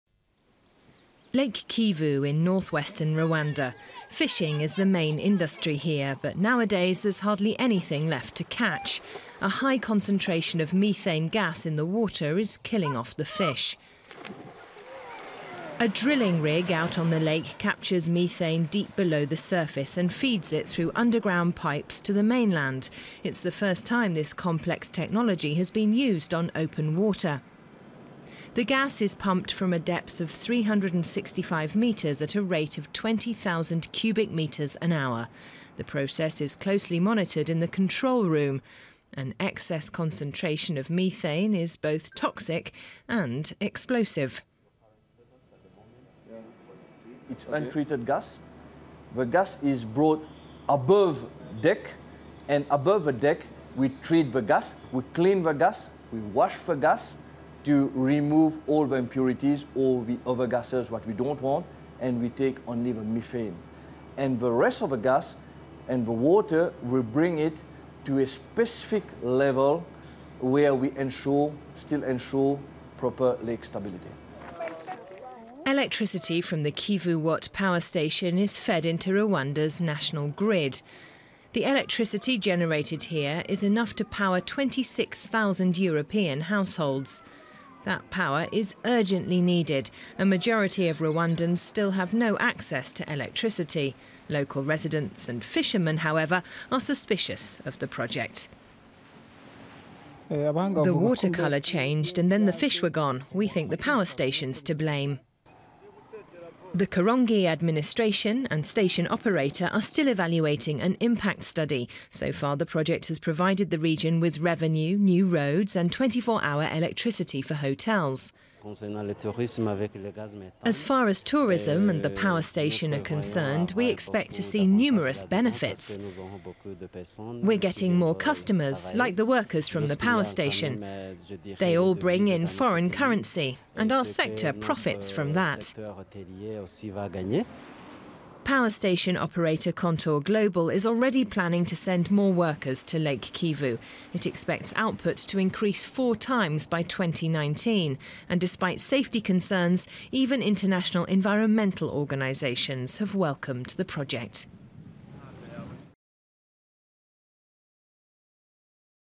Toxic lake turned power source in Rwanda | Business
Rwanda is the world's first country getting power from a lake - and averting a natural catastrophe at the same time. Rwanda's Lake Kivu contains a dangerously high concentration of methane gas due to nearby volcanic activity. Extracting the gas could save the lake and provide an economic opportunity for the electricity-starved country.